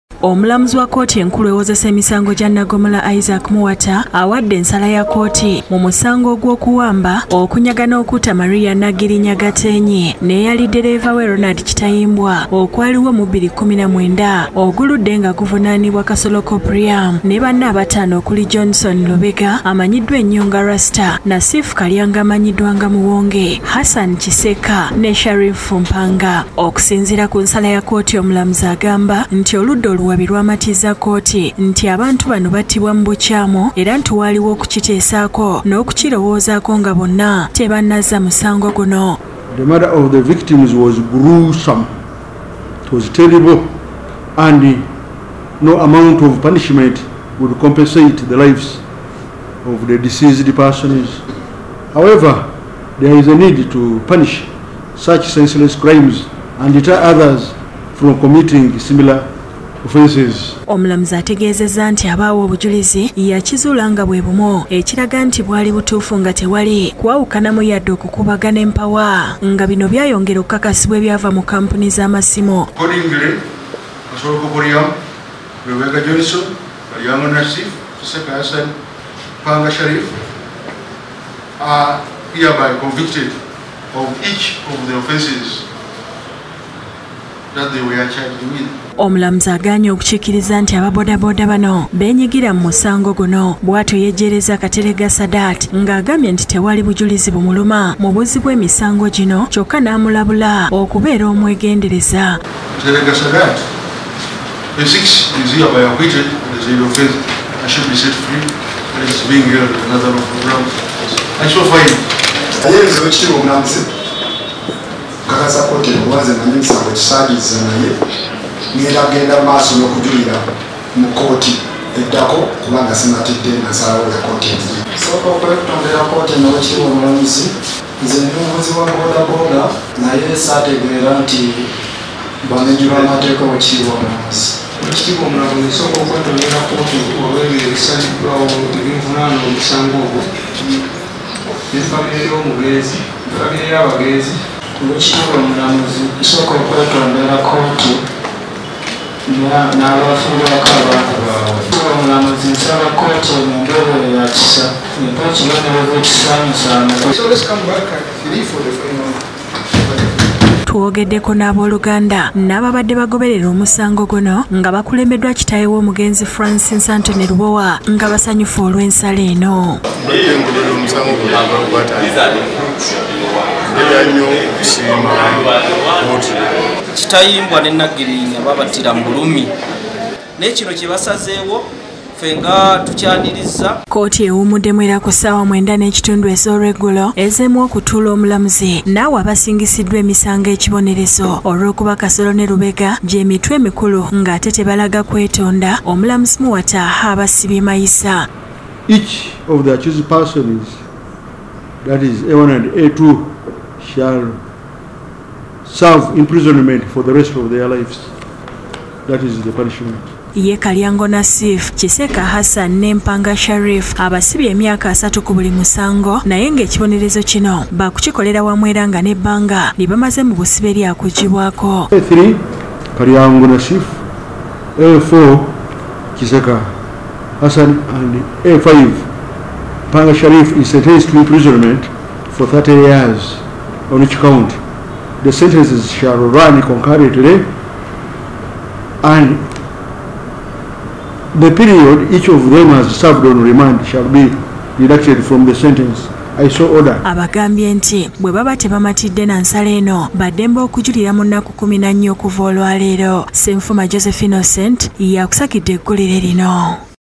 Agataliikonfuufu: KASOLO COMPULIYAMU ASIBIDDWA MAYISA!.
Omulamuzi wa kkooti enkulu Isaac Muwata asibye mayisa  Kasolo Coporiyamu ne Lubega Johson  ku misango gy’okuwamba n’okutta Maria Gorreti Naggirinya ne Ddereevawe Ronald Kitayimbwa. Abalala abasatu omulamuzi abasibye emyaka 30 ku buli musango nga ye omuwawabirwa ow'omukaaga  Kateregga Sadat amwejerezza.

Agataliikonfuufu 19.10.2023